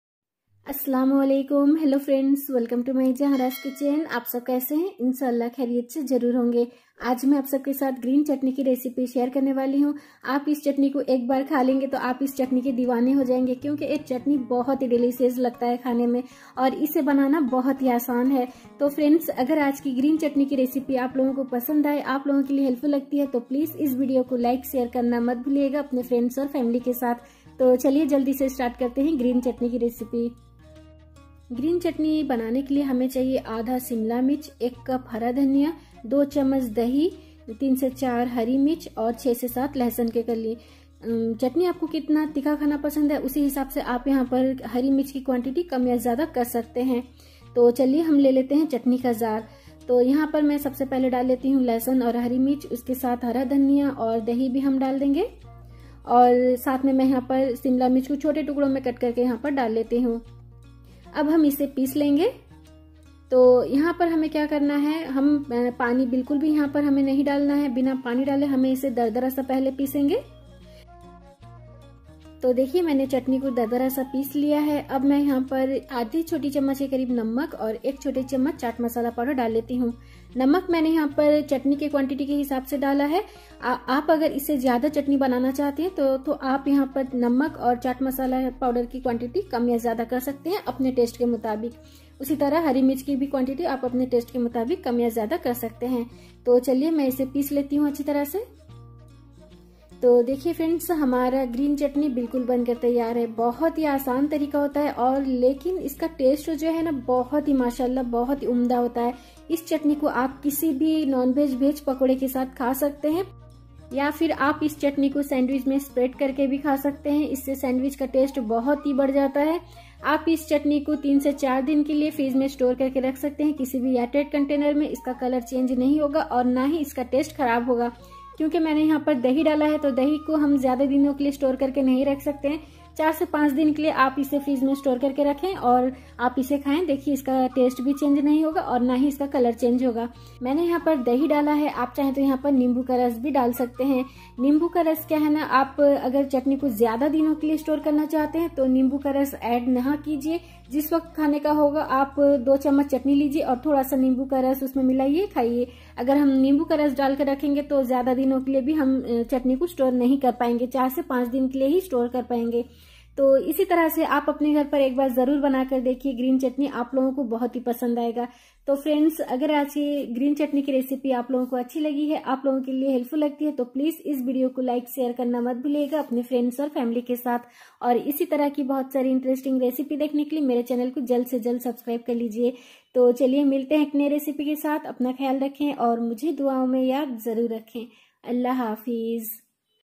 Green Chutney | Hari Chutney For Snacks Recipe | How to make Green Chutney Recipe in Hindi
TAKE CARE
STAY HAPPY
STAY BLESSED
BYE